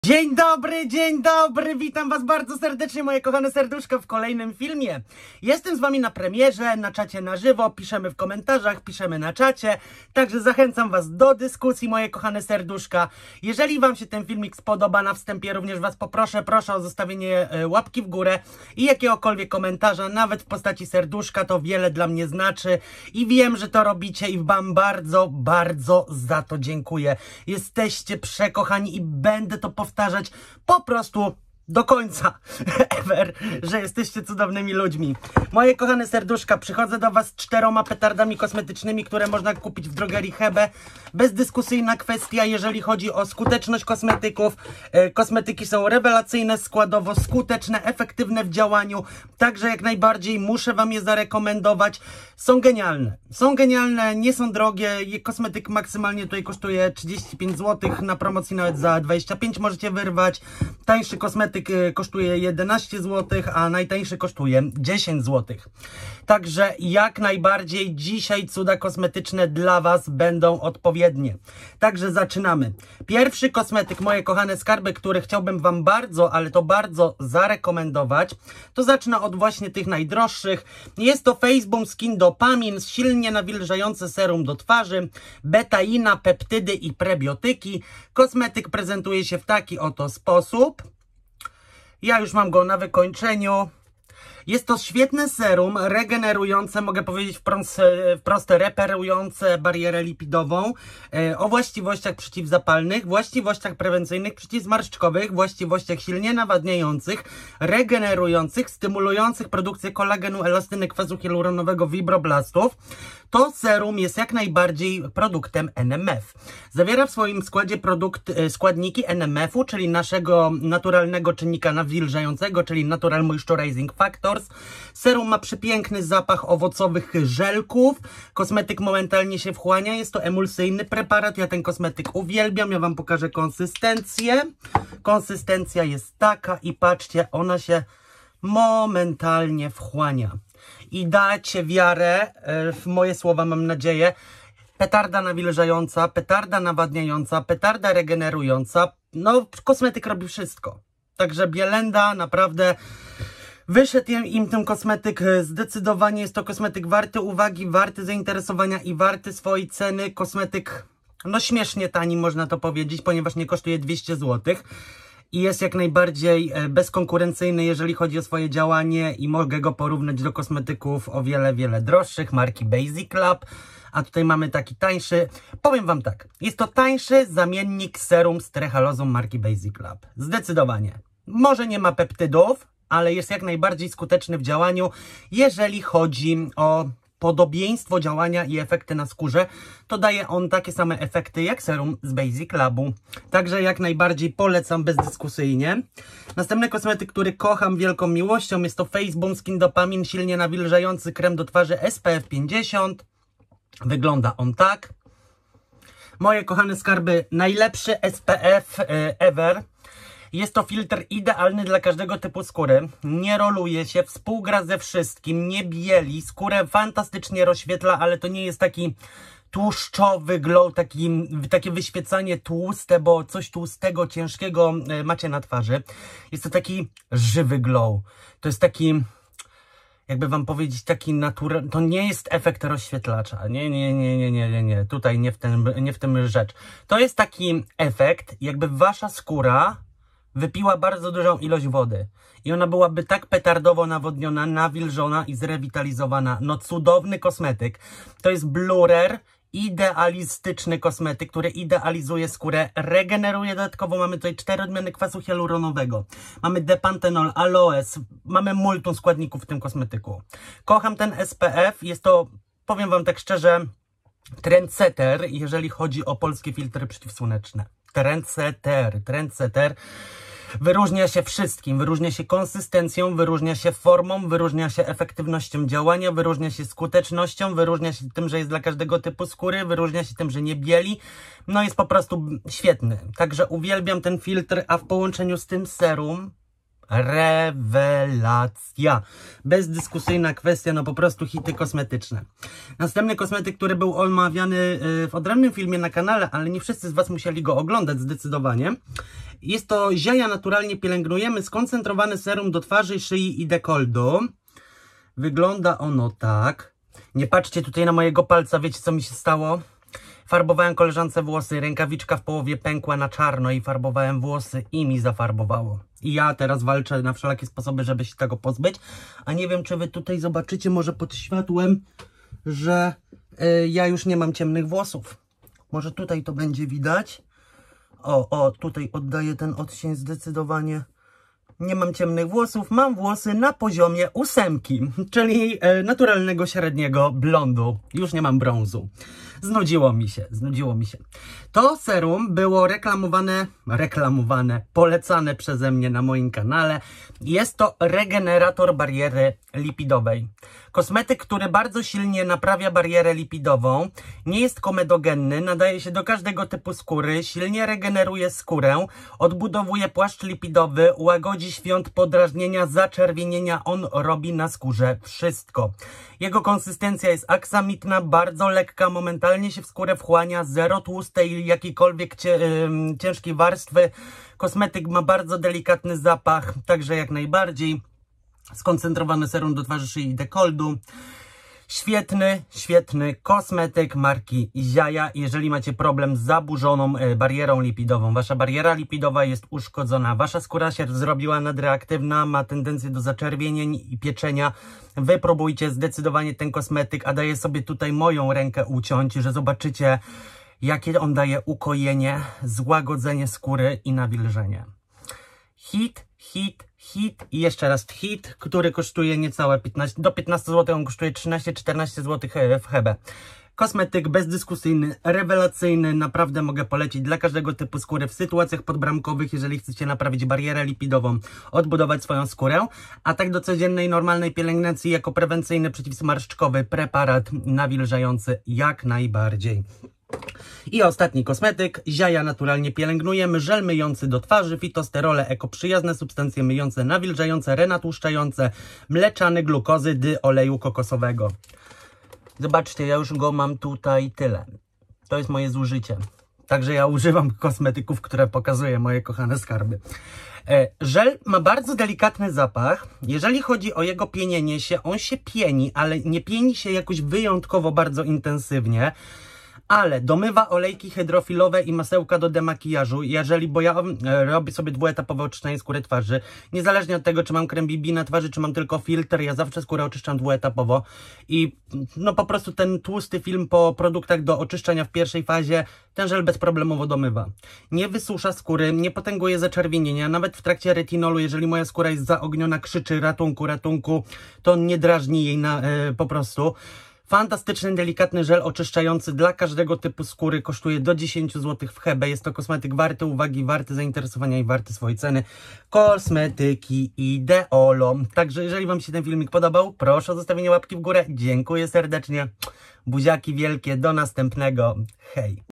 HEBE - WIELKIE TANIE CUDA!
Dzień dobry Moje Kochane skarby witam was bardzo serdecznie w kolejnym filmie z cudami kosmetycznymi z drogerii Hebe jeżeli was ten film interesuje to zapraszam do oglądania.

Ziaja naturalnie pielęgnujemy oczyszczający żel do mycia twarzy￼
Faceboom dopamine serum silnie nawilżające
Faceboom dopamine krem spf 50
Ziaja naturalnie pielęgnujemy przeciwzmarszczkowe skoncentrowane serum do twarzy nawilżające z fitosterolami￼￼